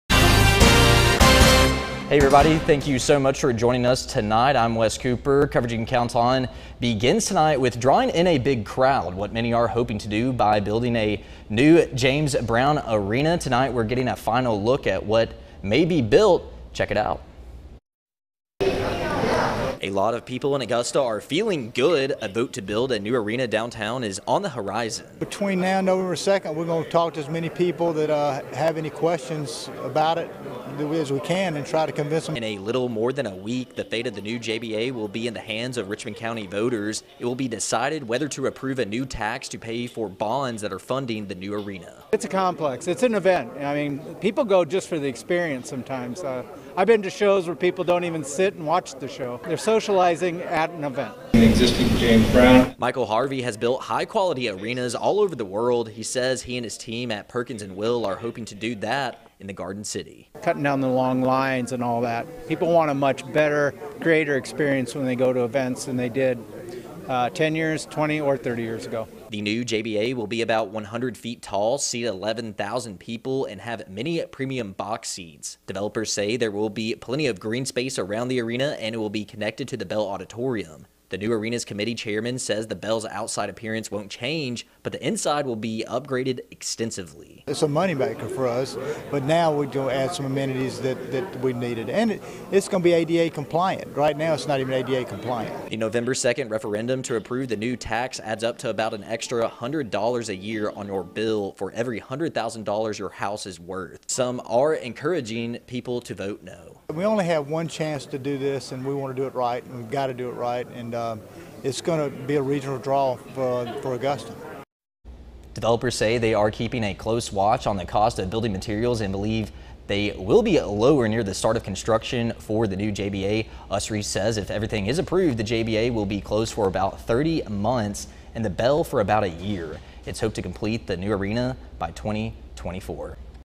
Final design for the new James Brown Arena revealed as referendum vote looms
Final design for the new James Brown Arena revealed as referendum vote looms by Wes Cooper